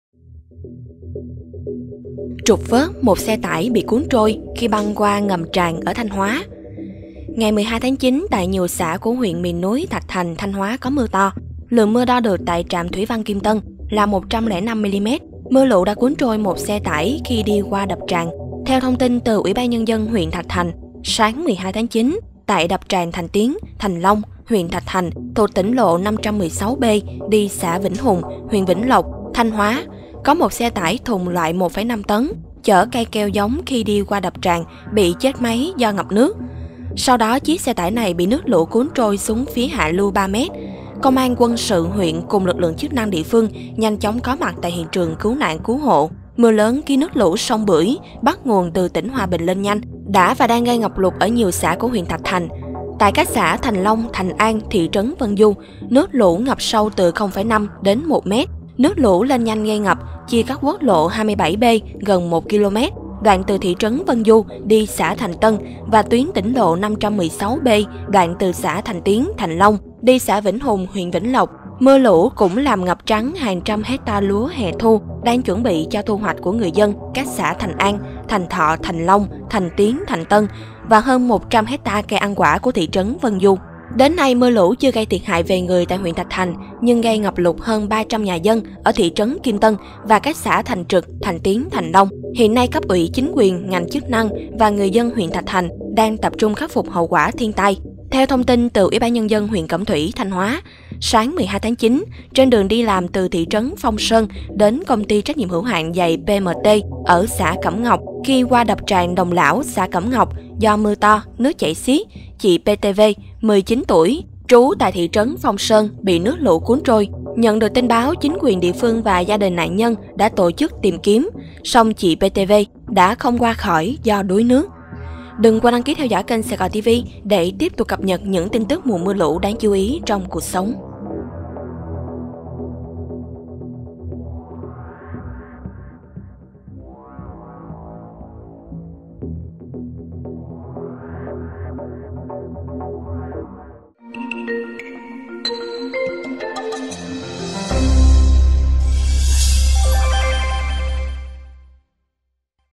Trục vớt một xe tải bị cuốn trôi khi băng qua ngầm tràn ở Thanh Hóa | SaigonTV
Tại các xã Thành Long, Thành An, thị trấn Vân Du, nước lũ ngập sâu từ 0,5 - 1m. Nước lũ lên nhanh gây ngập, chia cắt quốc lộ 27B gần 1km, đoạn từ thị trấn Vân Du đi xã Thành Tân và tuyến tỉnh lộ 516B đoạn từ xã Thành Tiến, Thành Long đi xã Vĩnh Hùng, huyện Vĩnh Lộc.